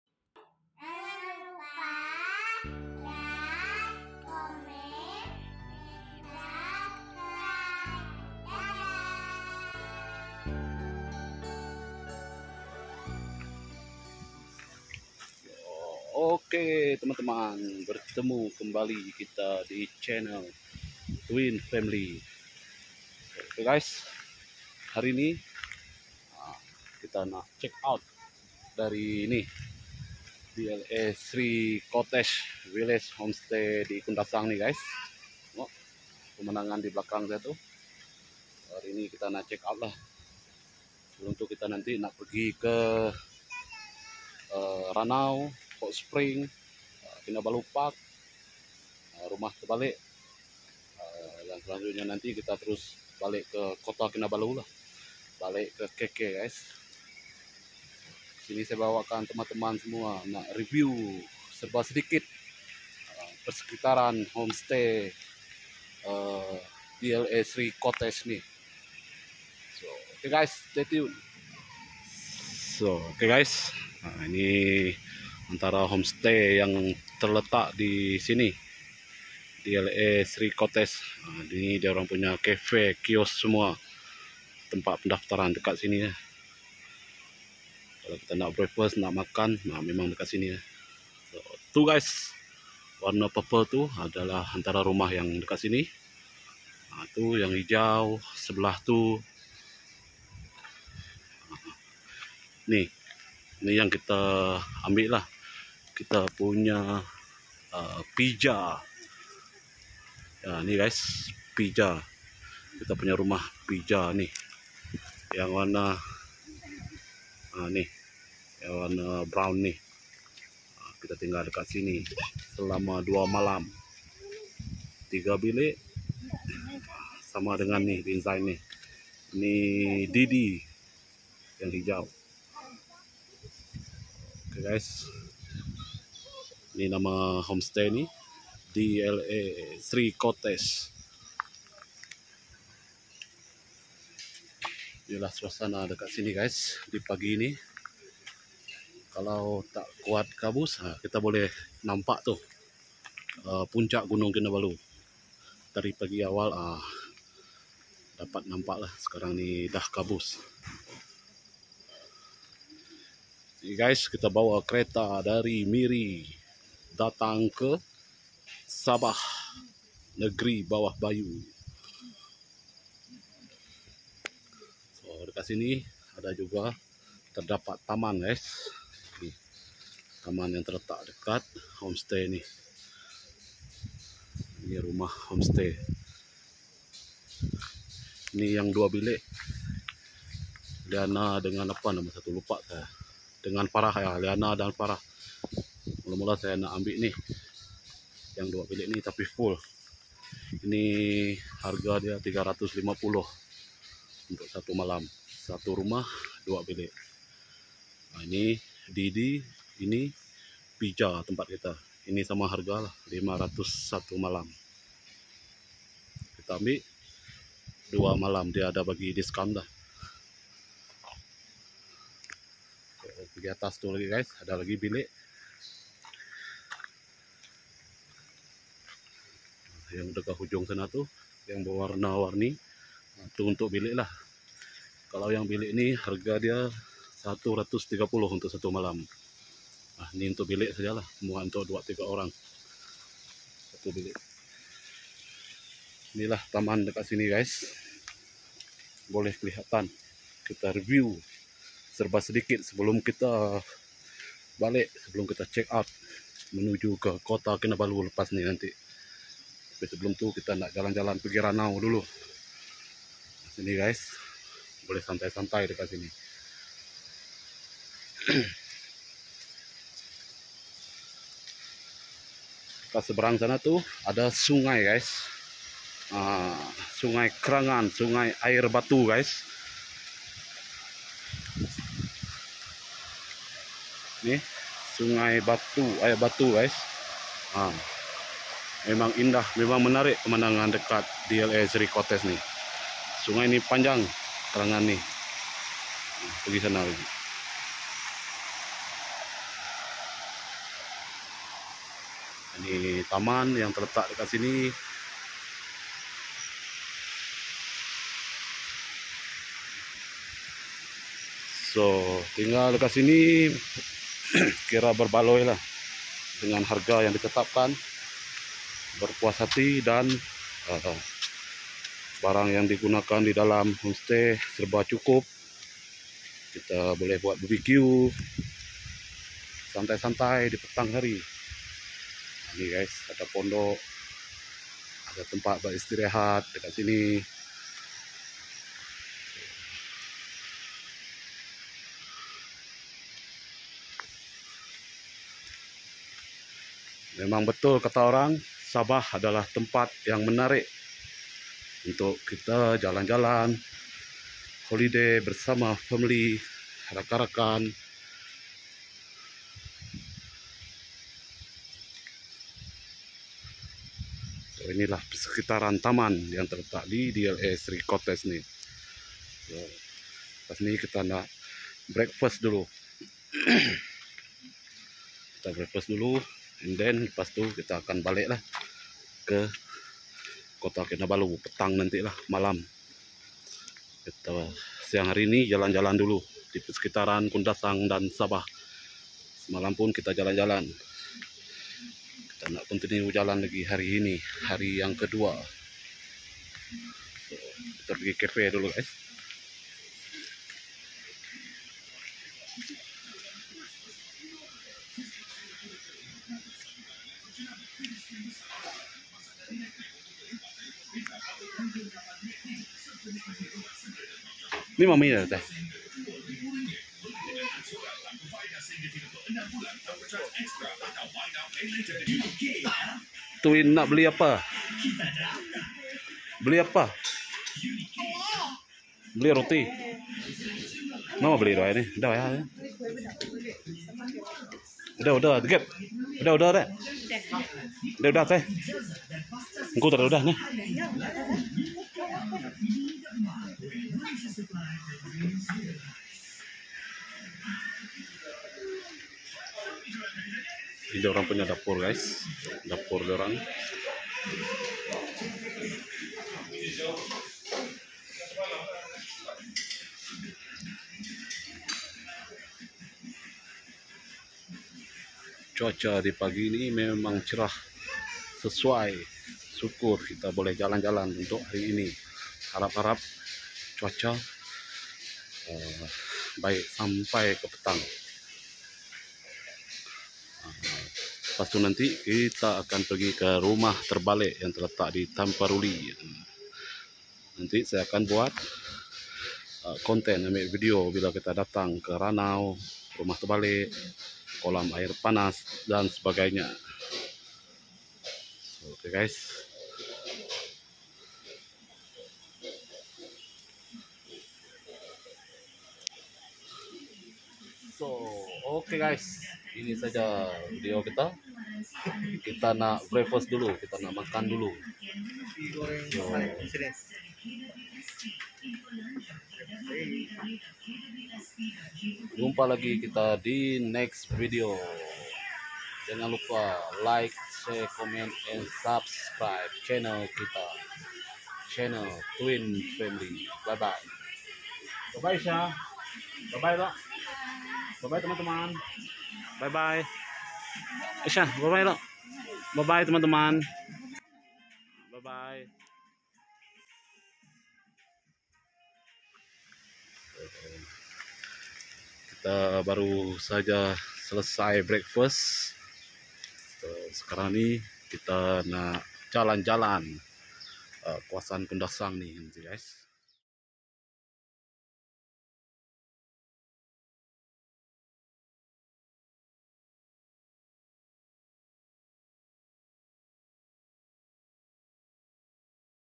D'LA Sri Cottage Kundasang,Tempat Penginapan Kami Sekeluarga Bercuti Di Sabah
bercuti bersama keluarga di sabah,tempat yang menarik untuk tarikan pengunjung adalah di kundasang